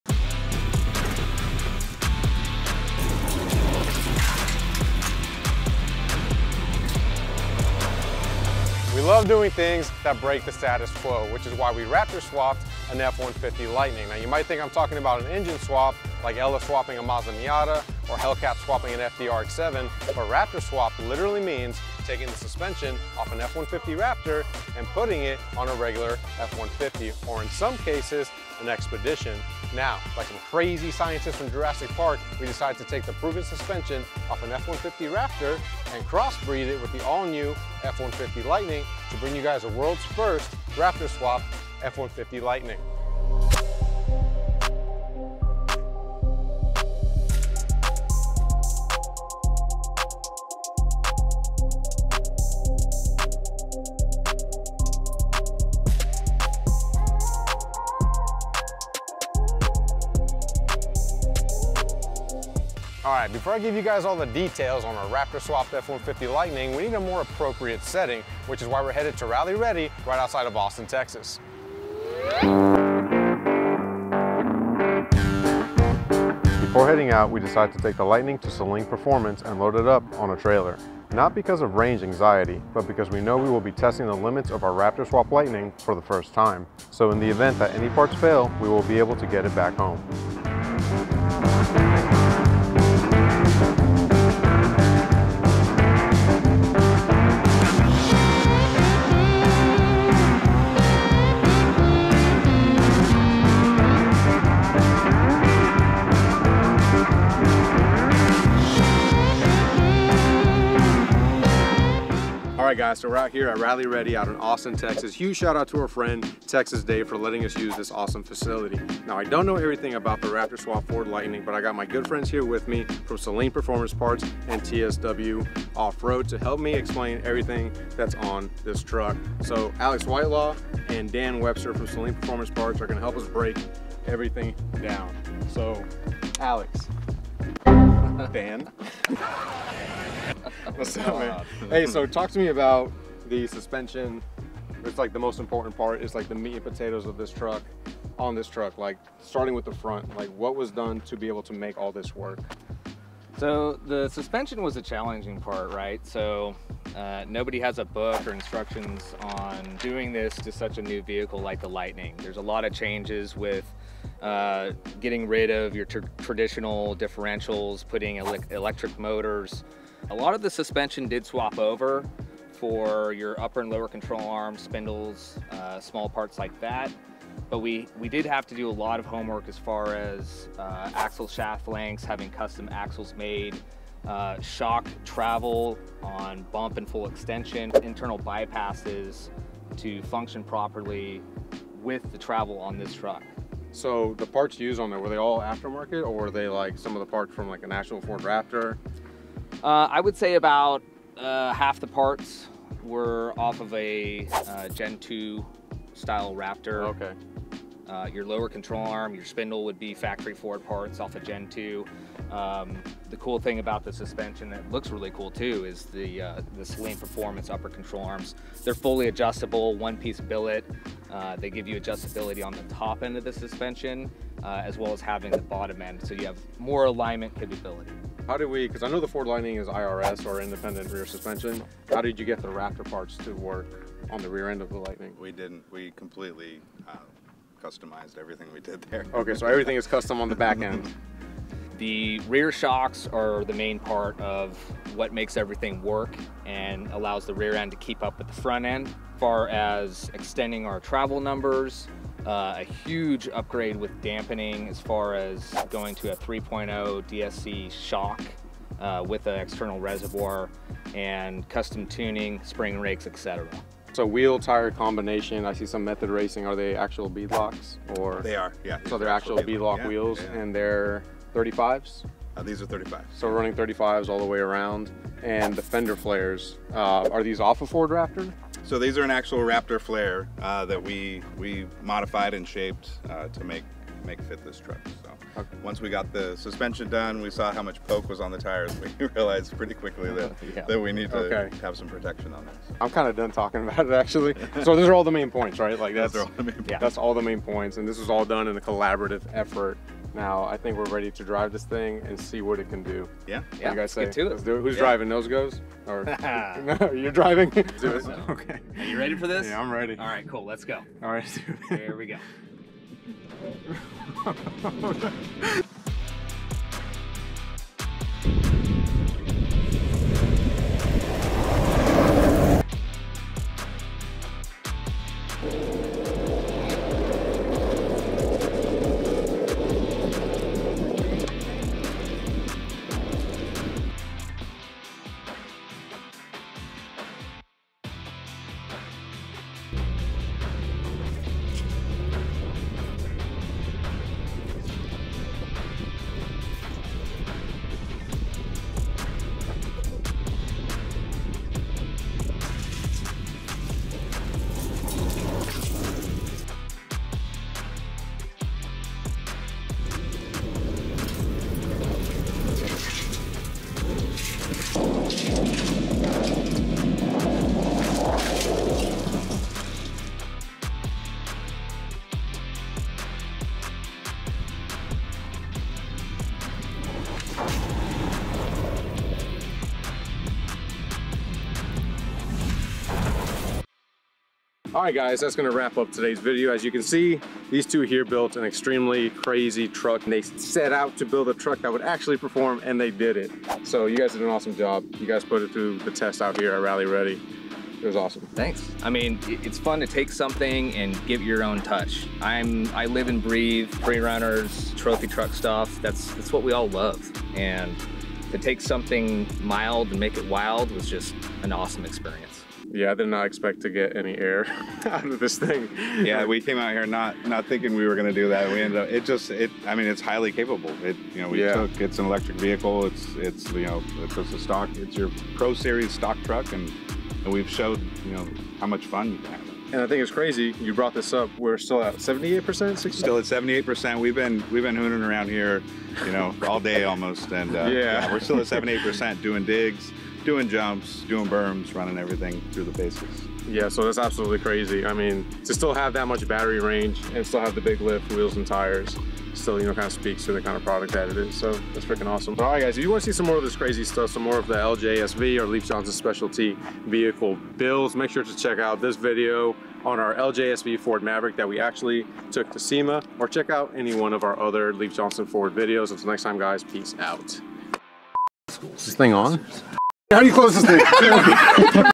Testing Our Raptor Swapped Suspension Ford F-150 Lightning!
In today's video we go over our Raptor suspension swapped Ford F-150 Lightning and take it off road on its first test run at Rally Ready to test the power of the machine of this off-road electric truck! Do you think the all electric Ford F150 Lightning has the potential to wear the F150 Raptor name? How do you get one for your own? Then check out Leif Johnson in Austin, Texas!